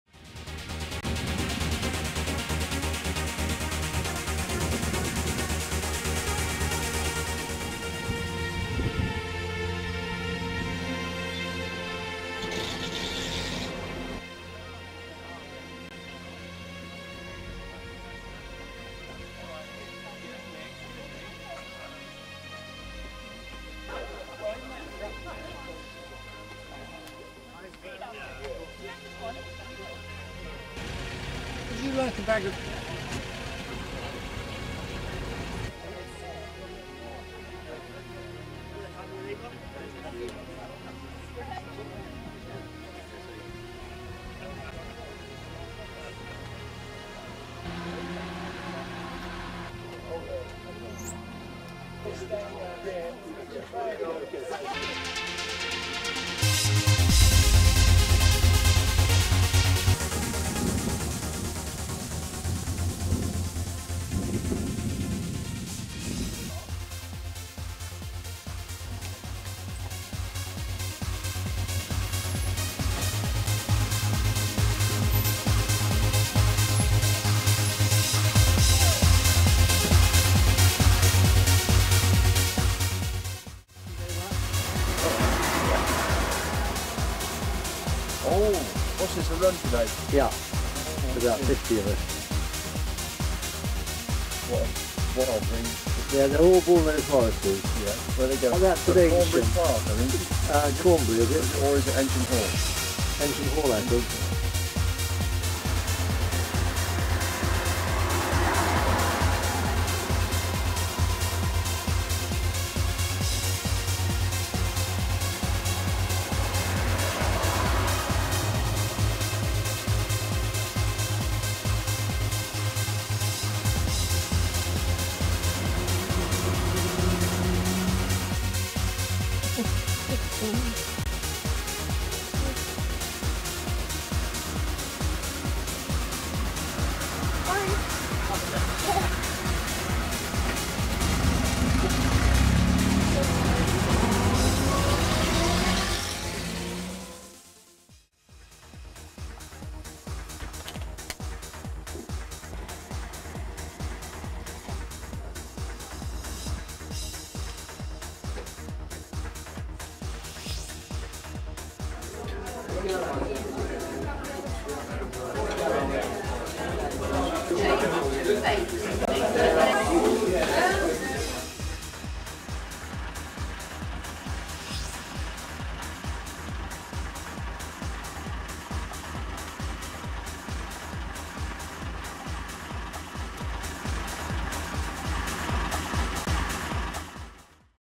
Bullnose Morris Club Rally, Minster Lovell, Oxford Autumn 2003
Highlights of the Club's Rally from the 29th August to 1st September 2003 where 55 Bullnose and Flatnose cars, built between 1913 and 1930, visited Prescott Hill Climb, Central Oxford and Hook Norton and had to pay a fortune (5p) to cross the Swinford Toll Bridge!